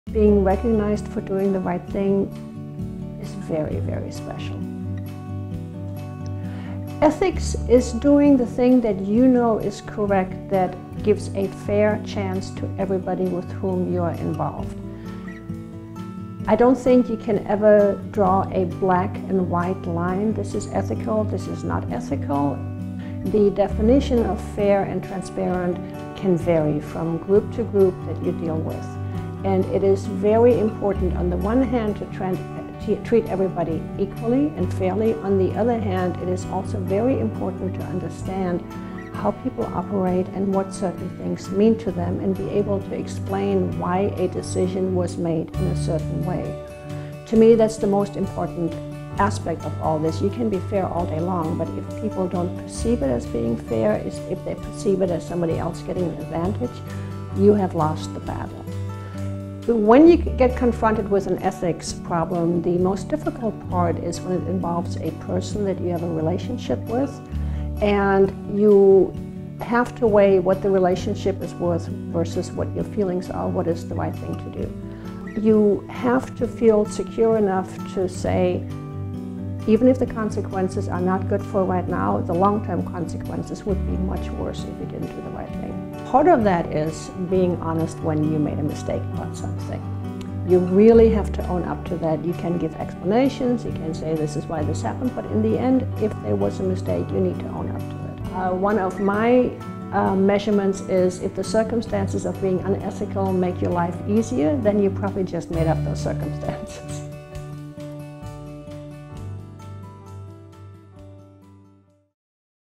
2016 Ethics in Business & Community Awards - Ingrid Weigand
The Ethics in Business & Community (EiBC) Award is given annually to businesses, nonprofits, and individuals who exemplify ethical practices in their official dealings and day-to-day endeavors. To find out more about the awards,